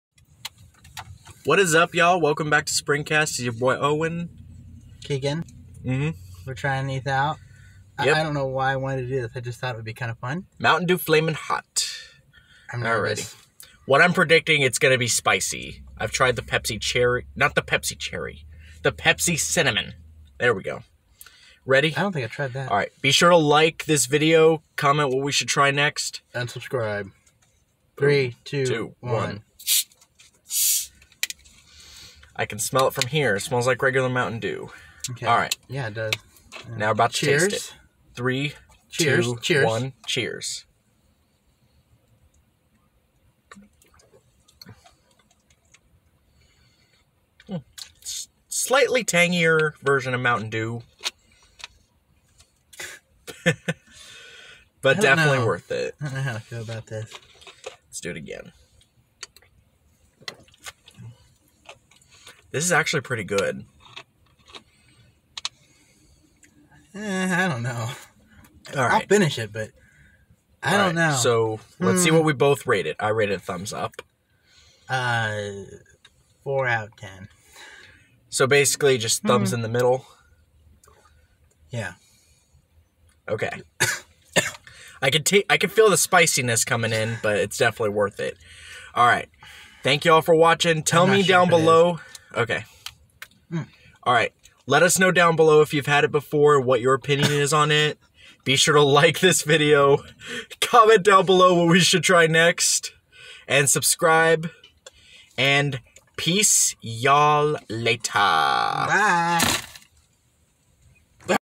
First Taste: Flamin' Hot Mountain Dew (IS IT HOT OR NOT)
Today, Keegan and Owen try the brand new Mountain Dew Flamin' Hot flavor, let's see how they do